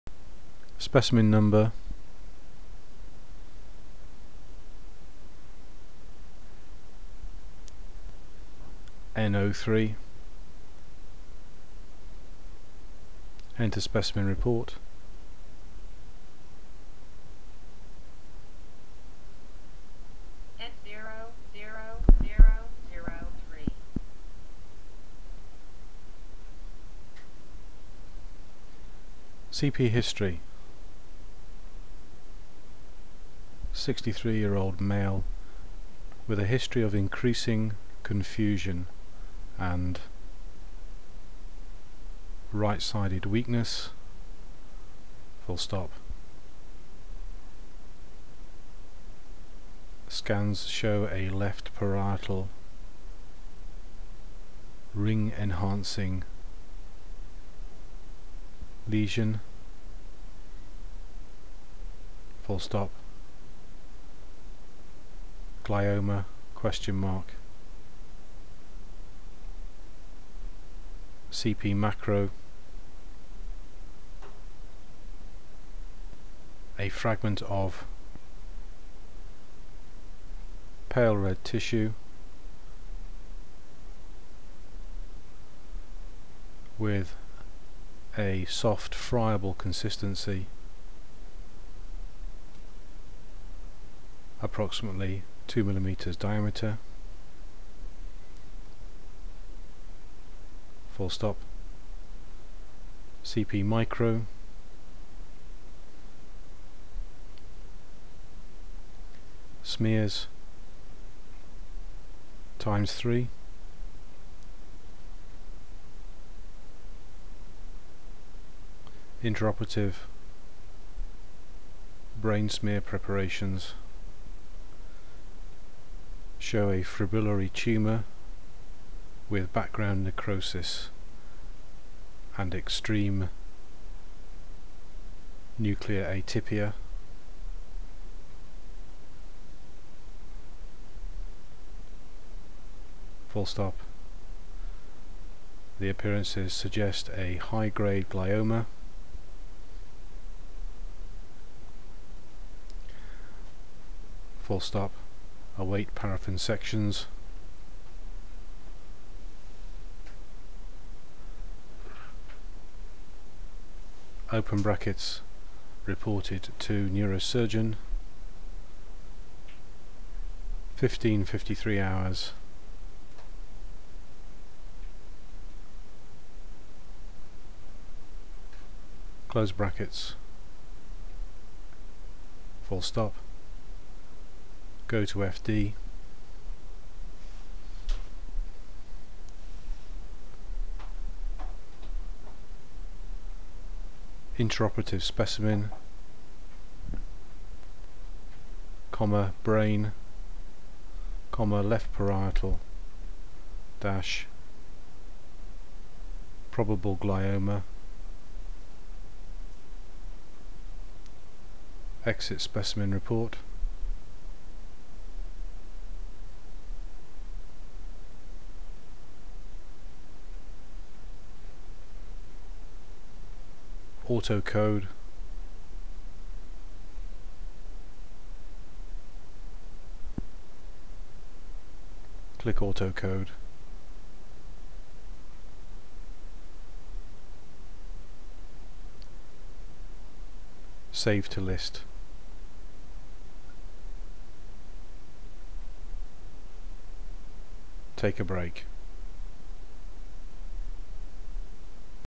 Dragon Speech Recognition with CoPath.avi
Demonstrates speech recognition being used directly with the Pathology clinical package CoPath, automation of workflow via voice commands can also be observed.